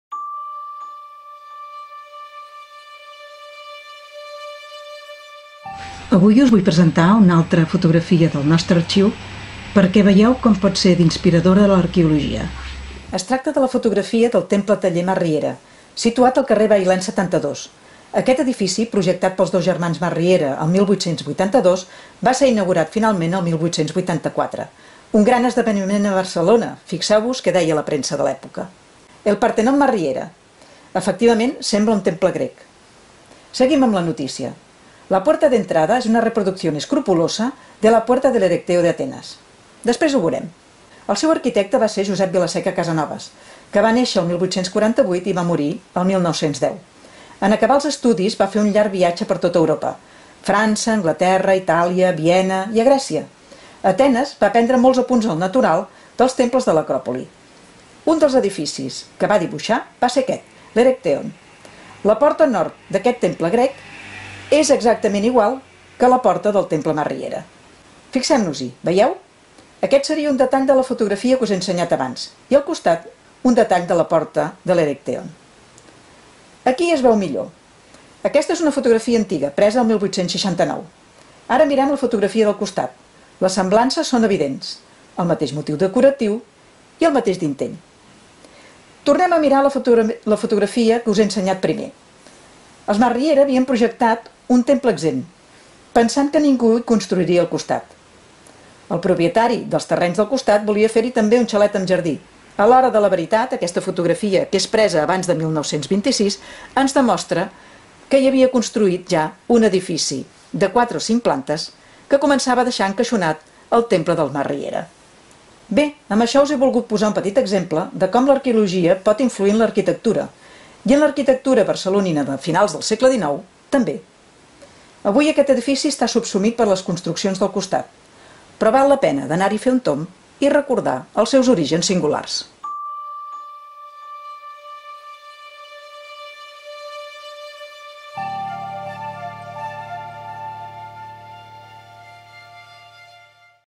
MuseuEndins - El partenó Masriera
La conservadora Àngels Casanovas ens descobreix la inspiració d'un curiós edifici de Barcelona, el partenó Masriera.